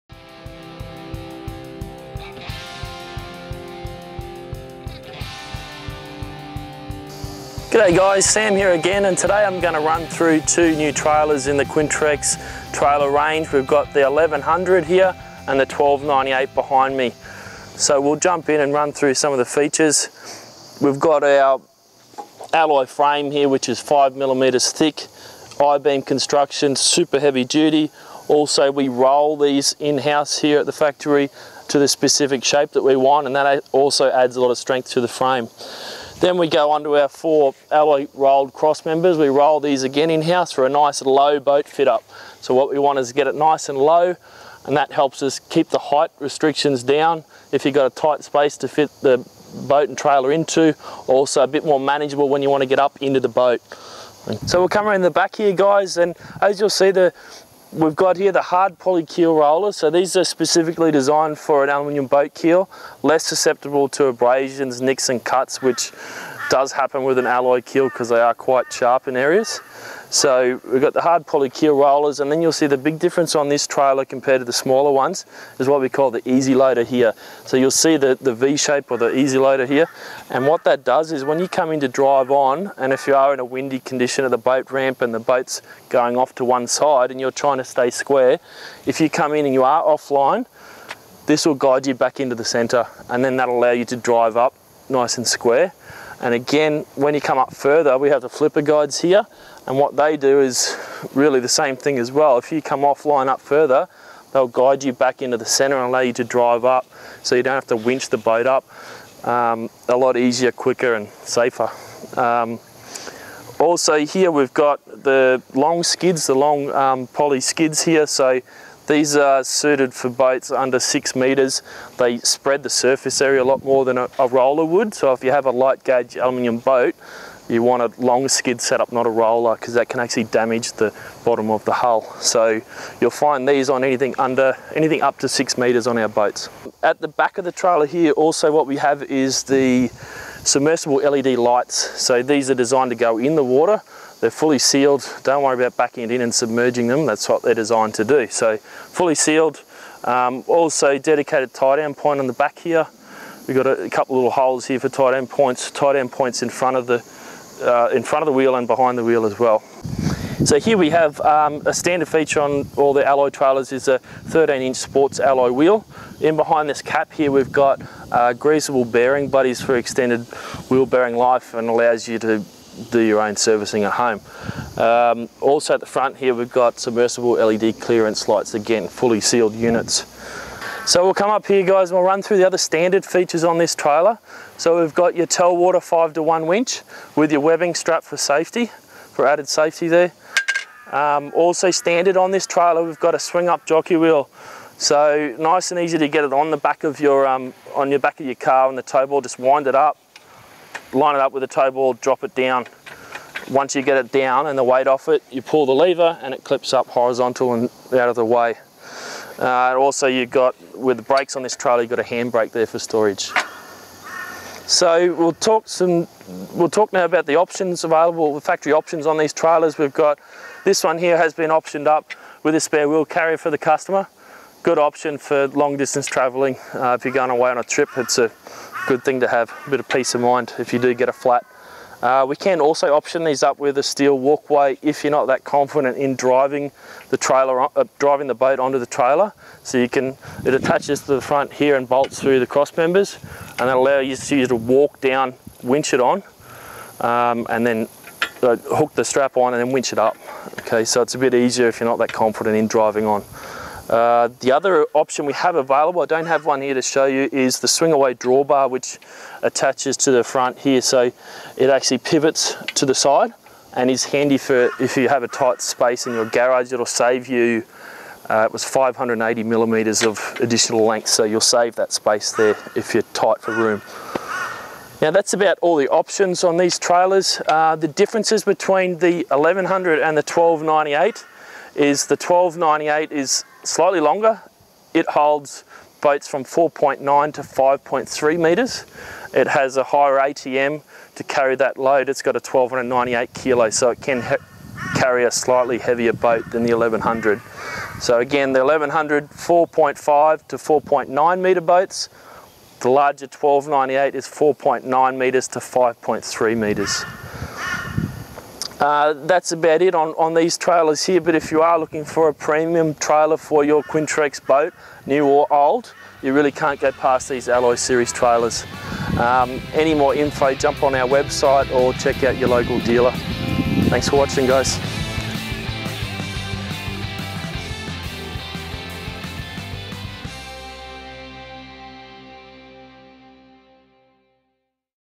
1100kg & 1298kg Quintrex Trailer
1100KG ALLOY TRAILER

Featuring the Easy Loader system and a swing jockey wheel this trailer makes loading and launching you boat a breeze.

1298KG ALLOY TRAILER

With standard drive on flipper guides for easy loading and a tough aluminium frame this trailer is ready to hit the boat ramp and you can even option up with a walkway and spare wheel.